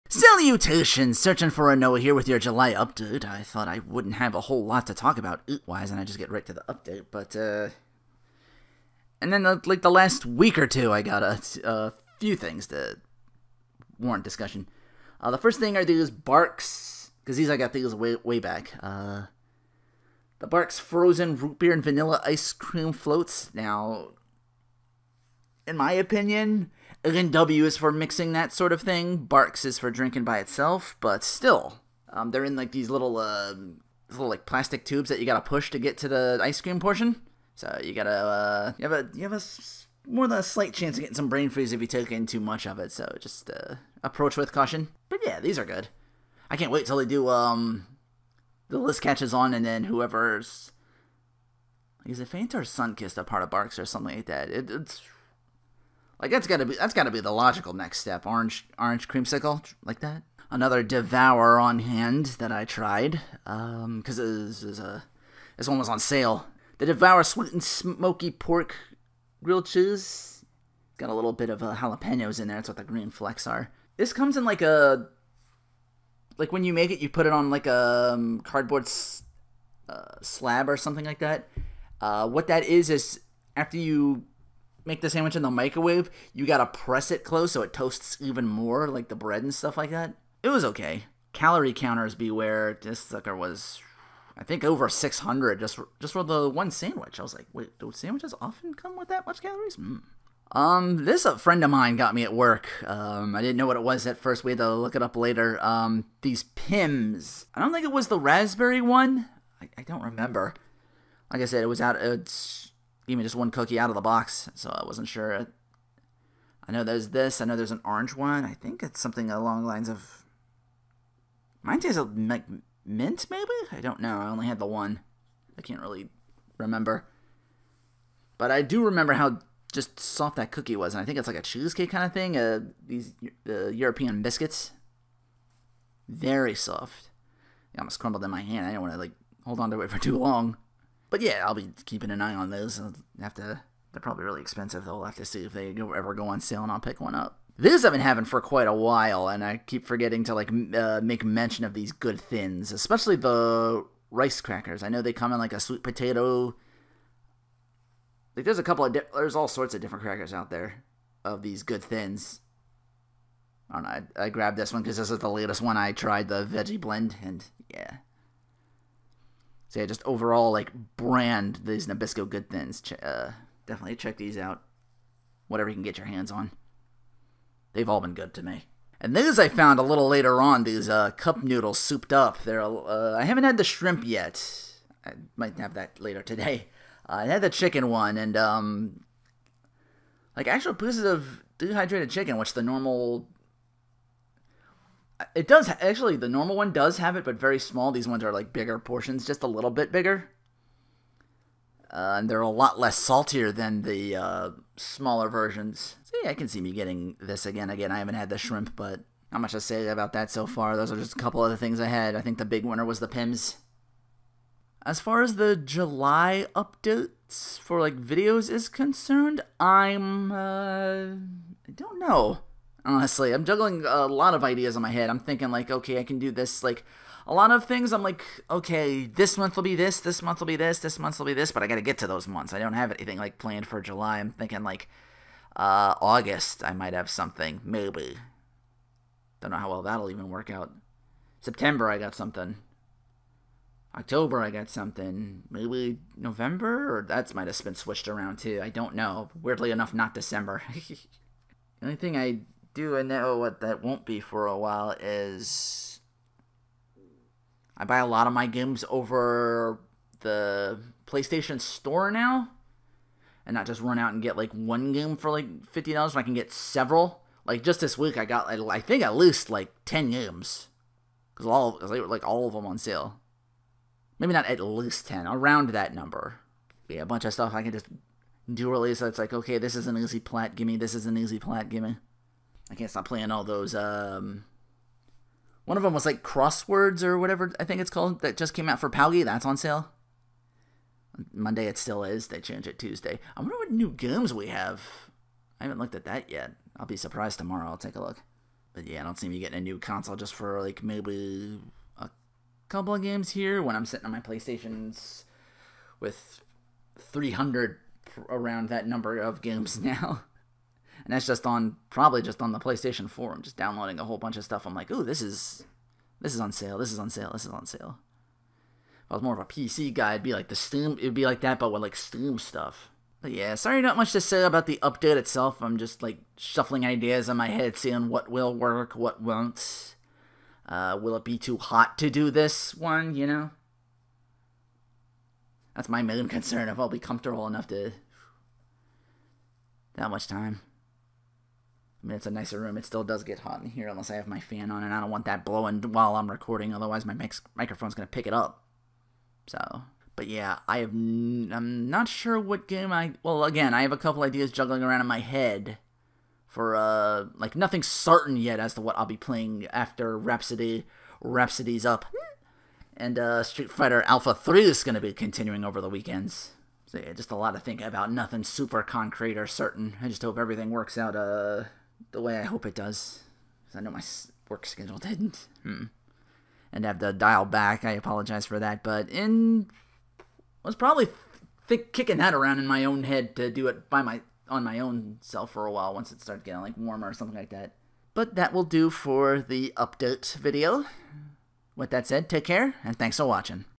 UPDATE VIDEO - July 2019 [Floating Ideas, Nothing Concrete]
And here I thought it'd just be an update video with maybe 1 or 2 eats. Now it's several eats with nothing SUPER concrete, and me just hoping things work out.